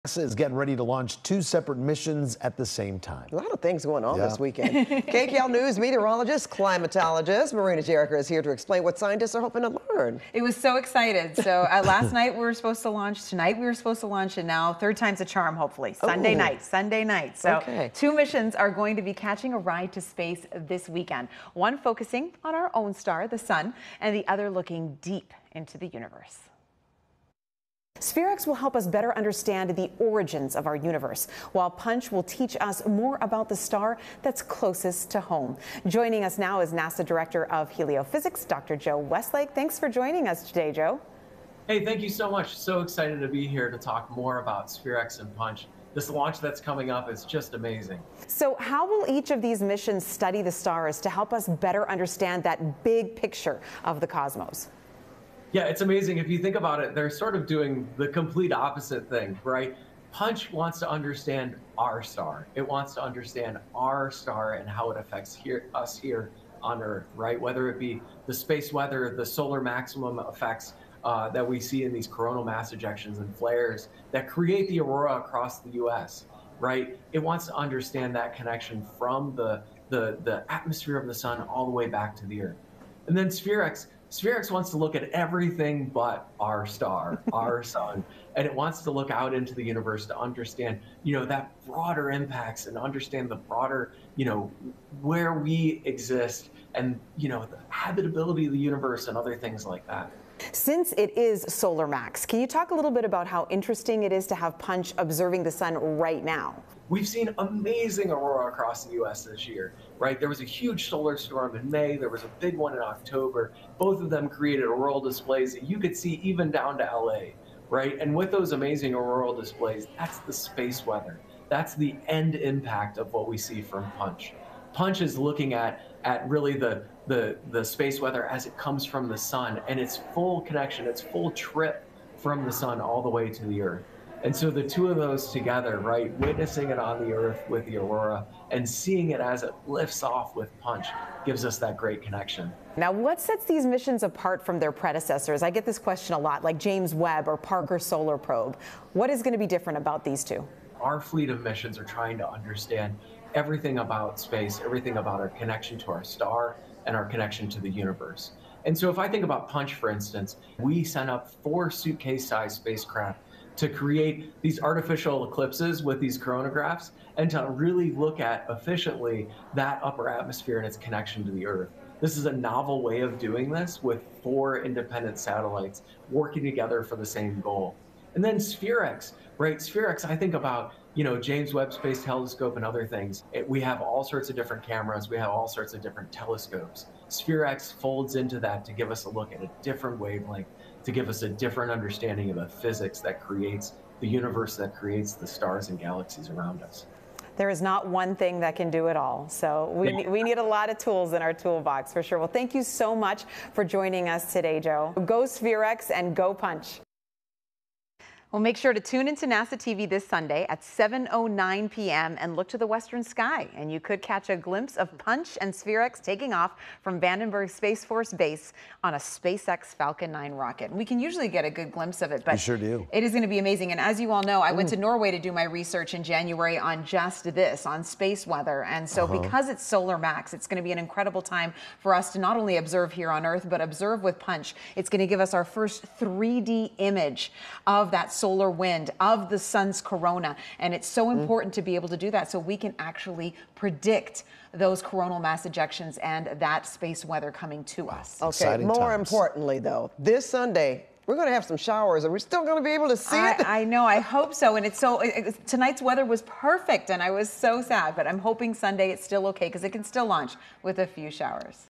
NASA prepares for two missions, one to the sun and another to deep space
Marina Jurica talks about the two missions NASA is preparing for.